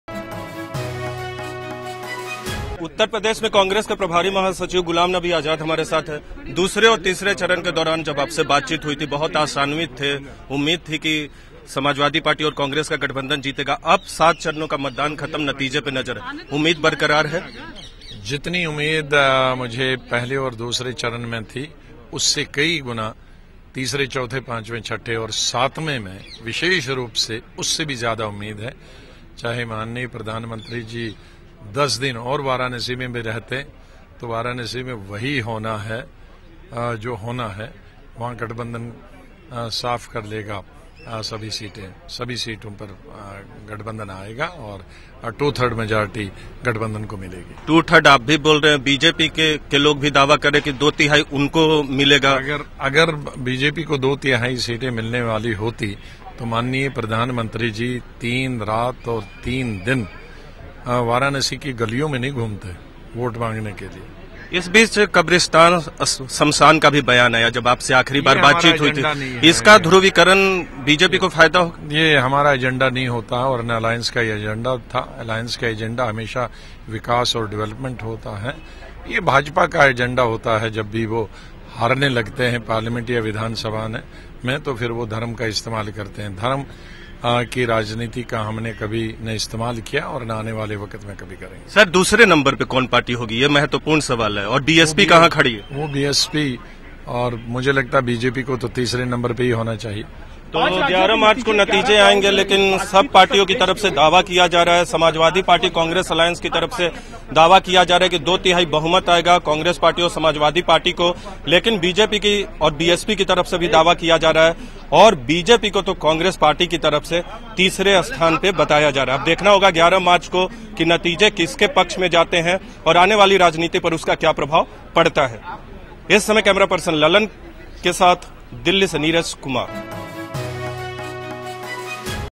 UP Congress in-charge Gulam Nabi Azad spoke on about Assembly polls of UP
UP Congress in-charge Gulam Nabi Azad spoke on about Assembly polls of Uttar Pradesh...